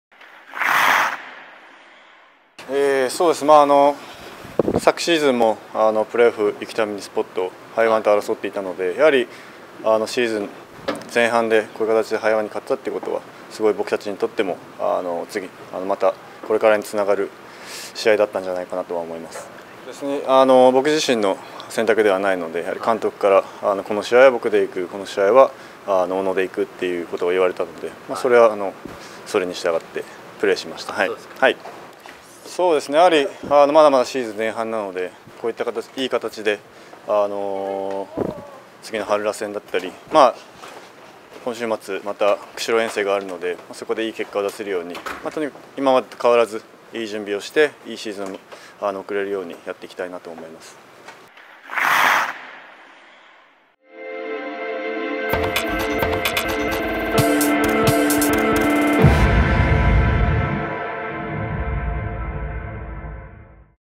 INTERVIEW | FUKUFUJI Yutaka | 2016. 10. 22 | High1 vs Nikko Icebucks
INTERVIEW
FUKUFUJI Yutaka
High1 vs Nikko Icebucks
Asia League Ice Hockey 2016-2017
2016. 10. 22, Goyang